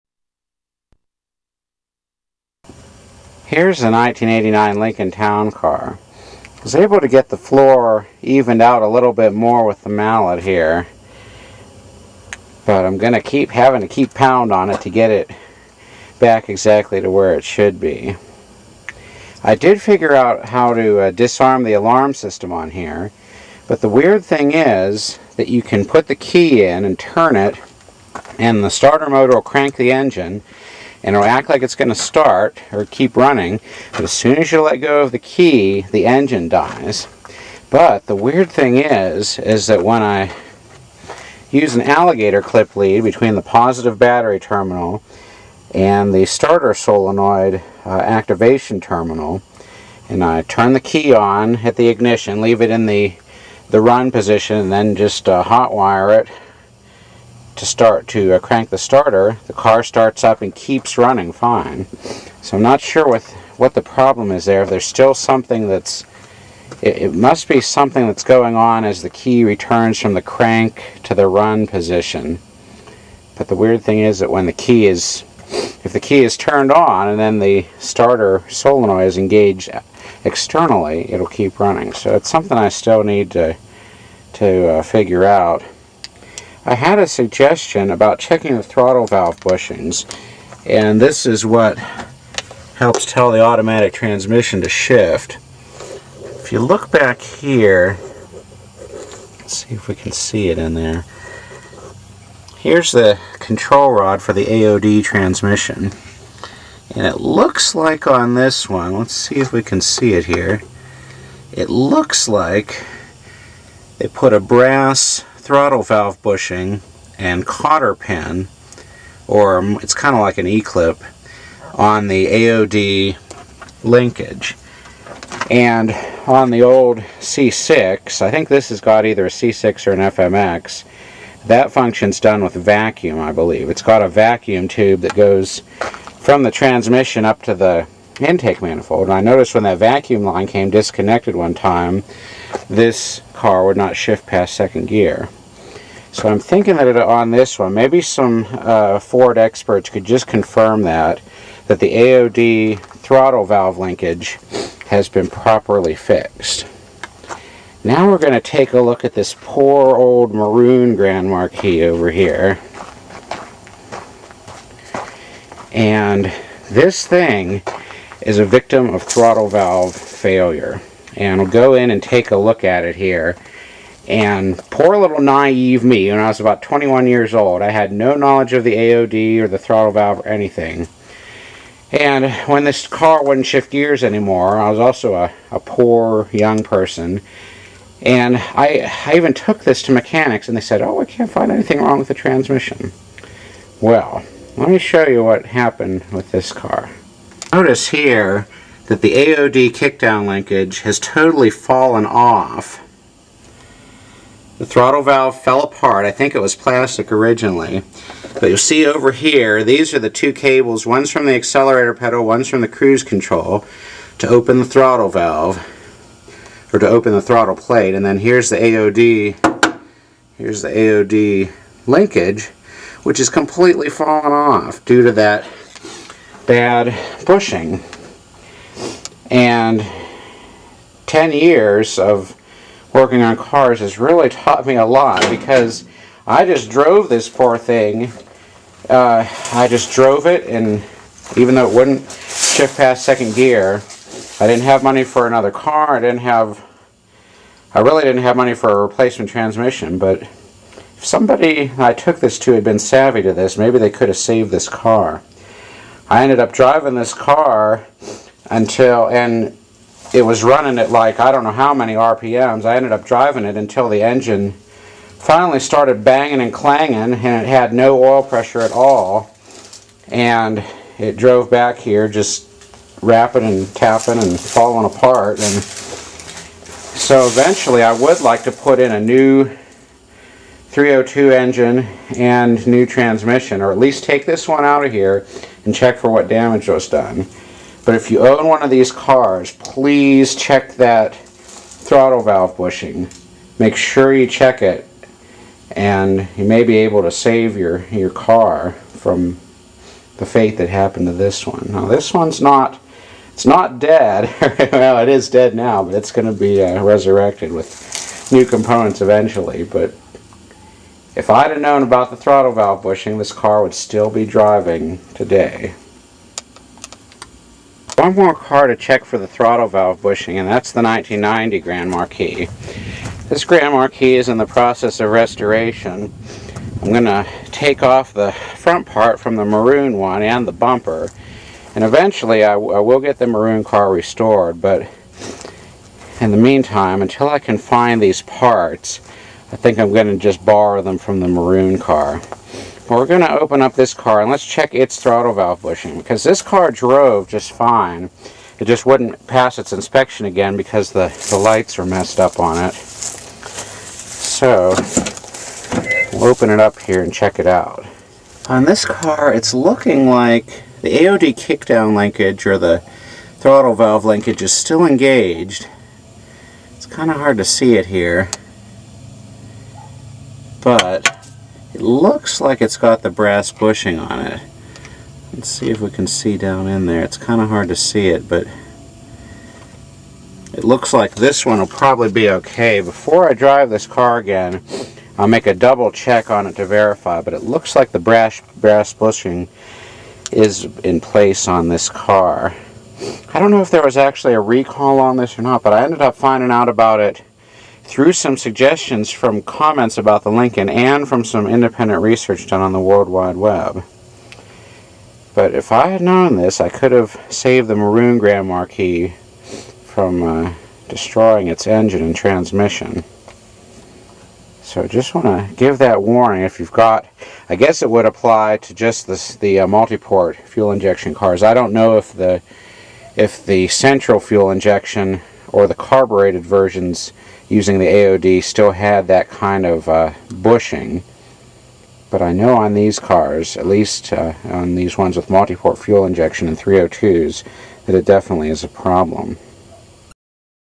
Ford AOD transmission throttle valve bushing check
I checked my 1989 Lincoln, and 1991 and 1990 Grand Marquis. The Lincoln's throttle valve cable was attached and looked in good shape, but I'm not 100% sure the brass bushing was installed. There is apparently a problem with the factory bushings where the disintegrate and fall off. The 1990 Marquis' cable also looked OK...the 1991 however was a victim of the throttle valve cable coming loose back in 2000 or so, causing the transmission to never shift past 2nd gear. At the time I didn't know about the TV cable problem.